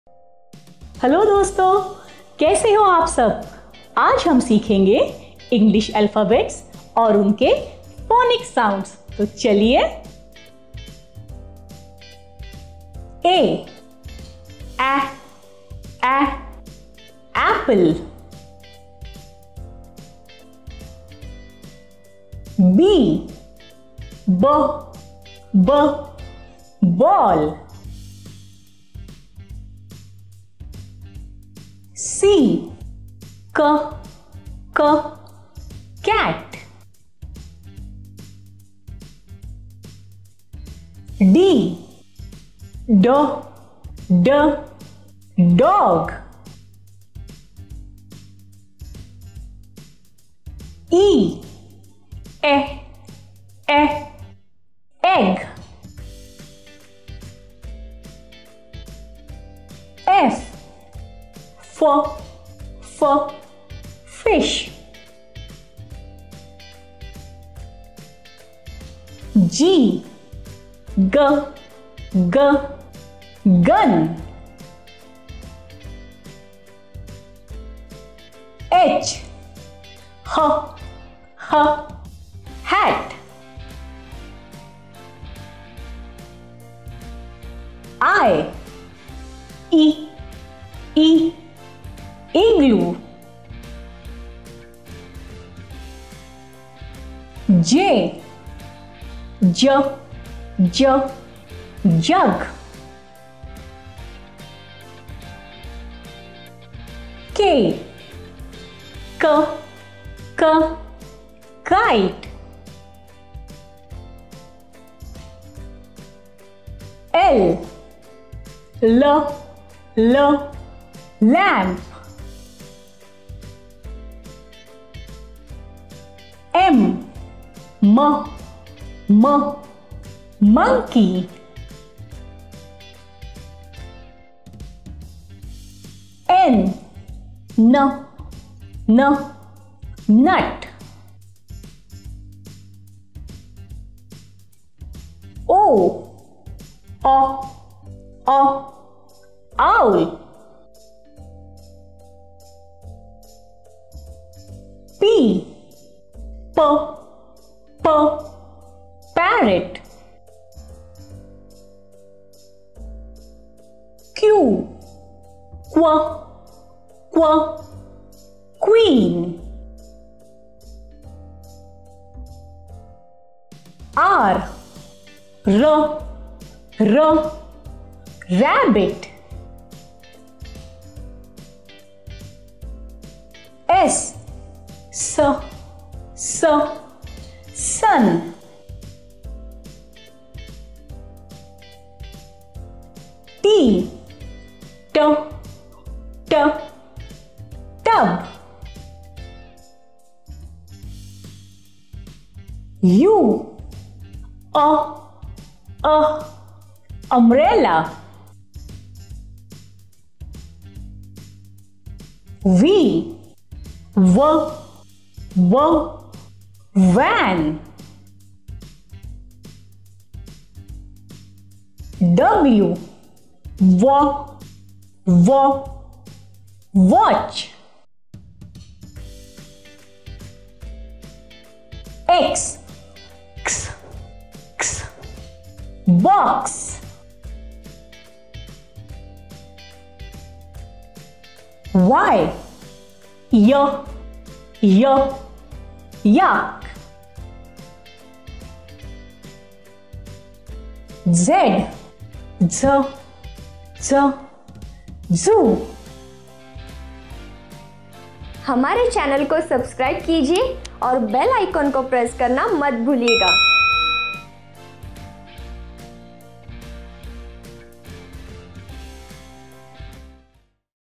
Phonics Sounds in Hindi | A to Z Alphabets with Phonics Sounds | School Learning | Pebbles Live
@Pebbles present, Phonics Sounds in Hindi.
A to Z Alphabets with Phonics Sound,
Learn Phonics through Hindi,
Sounds of English Alphabets,
Phonics Sounds with Pictures,
Phonics Sounds with Words

Teacher : Shaifali
Concept, Production & Post Production by : Milind Patwardhan